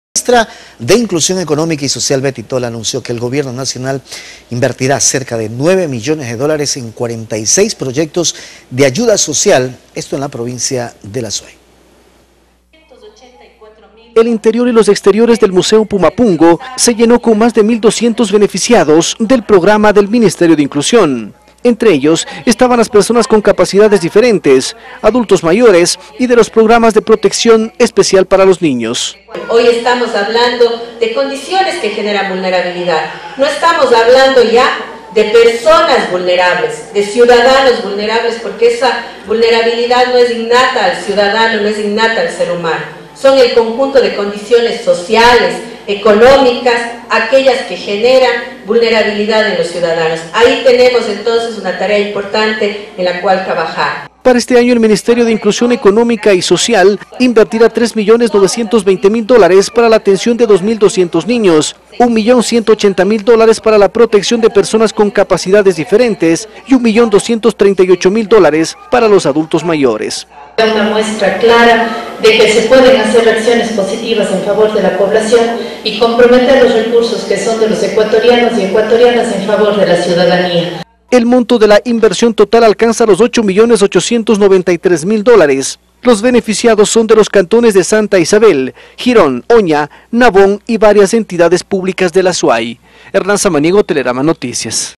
MIES aporta con cerca de 9 millones de dólares a proyectos sociales
La ministra de inclusión económica y social, Betty Tola, anunció que el gobierno nacional invertirá cerca de 9 millones de dólares,  en 46 proyectos de ayuda social en la provincia del Azuay...